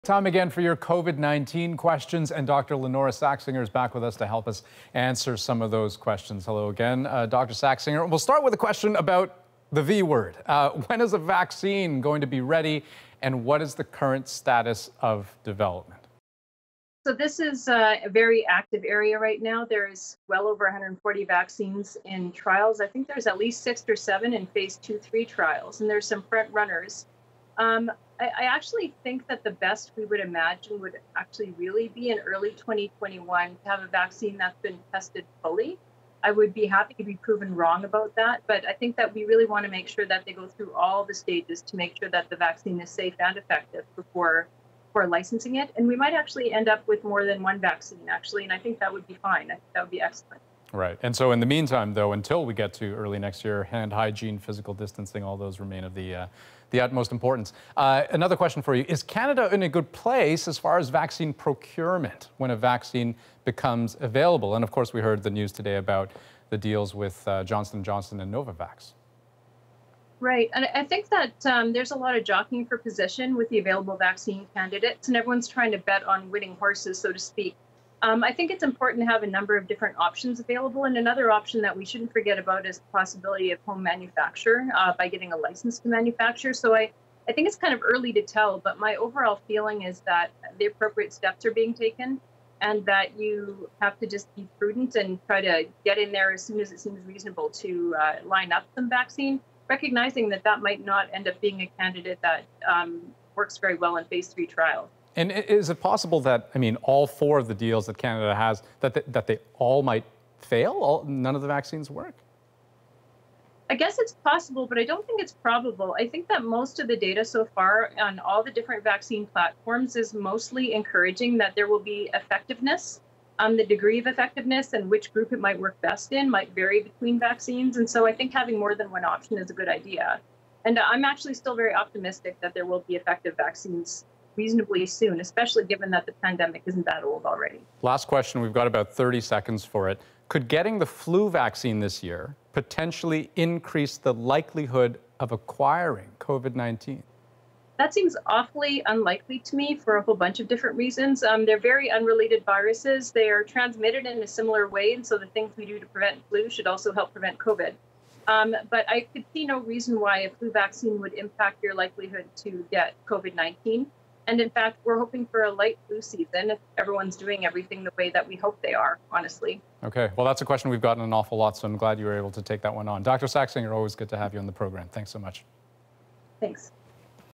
COVID-19: When will a vaccine be ready?
An infectious disease specialist answers viewer questions about a COVID-19 vaccine including what stage vaccine development is in and when the public could expect one to be ready.